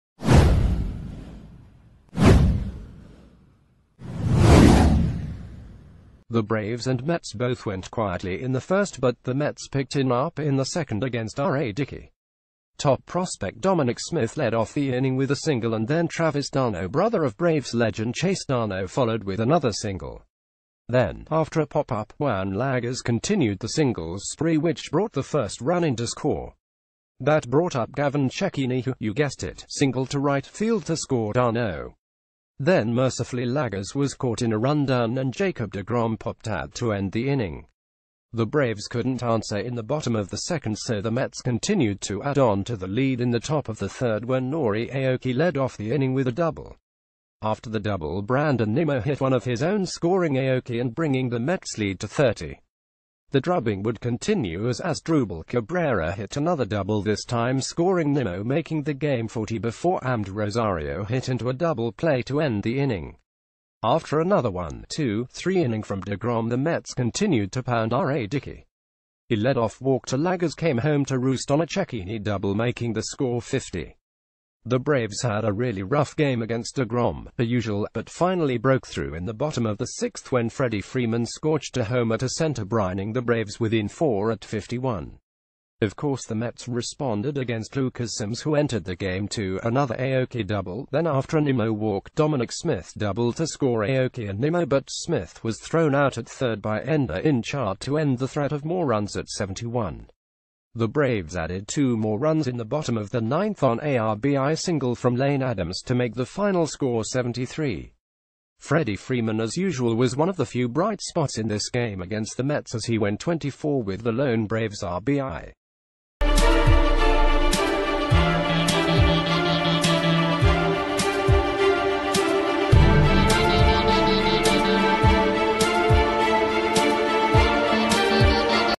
Breaking News | Braves can’t solve jacob degrom
Braves can’t solve jacob degrom

The Braves and Mets both went quietly in the first but the Mets picked in up in the second against R...